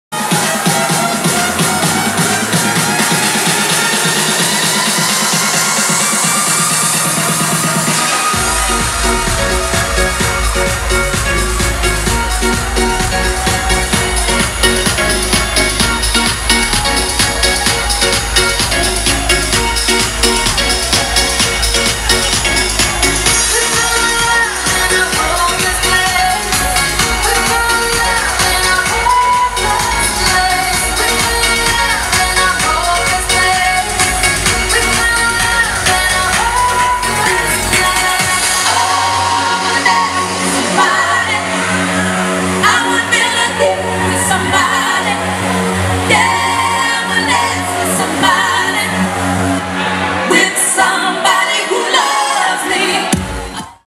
San Francisco DJ | DJ Jeremy | Sound Factory
DJ Jeremy in the mix at the Sound Factory in San Francisco, CA